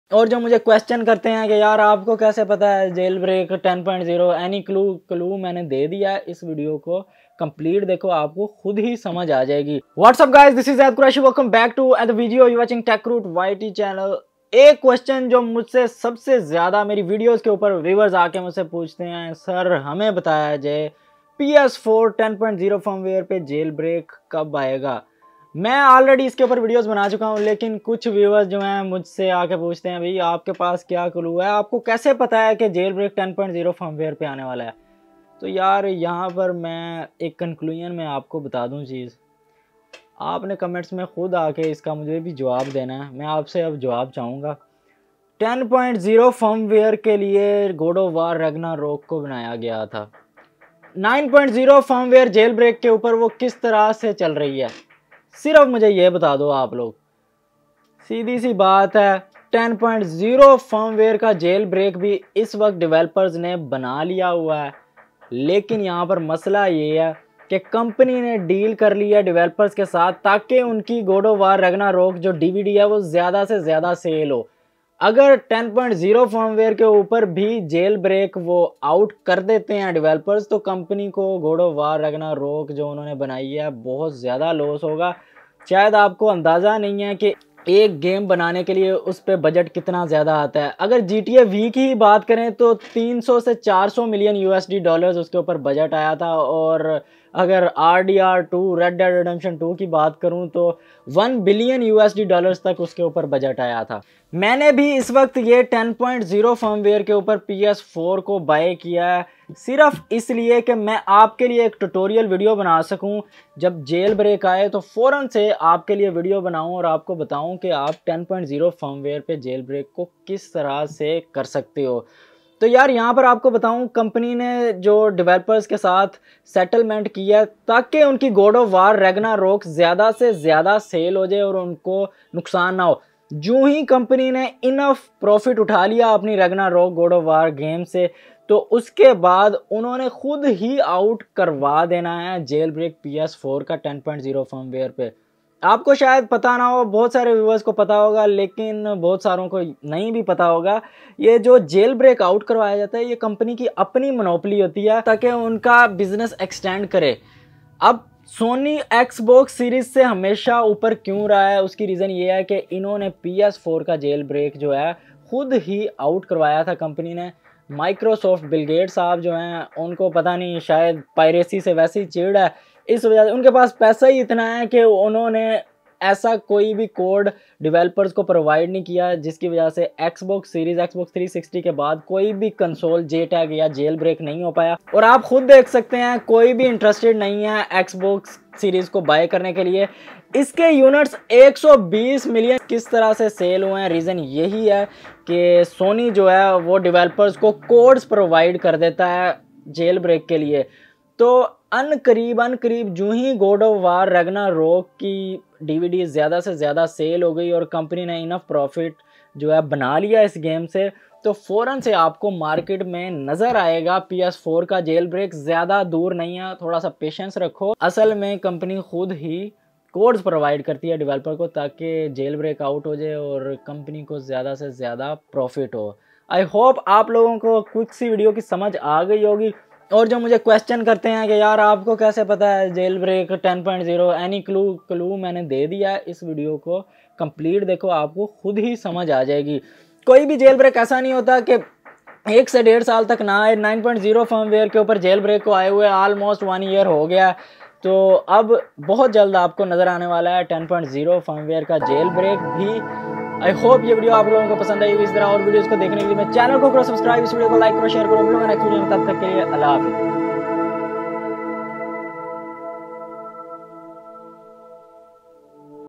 PS4 JAILBREAK On 10.0 Firmware Update | Clue Behind Jailbreak announcement
This video is about latest clue behind ps4 Jailbreak on Firmware 10.0 . Before Commenting on this topic please watch whole video.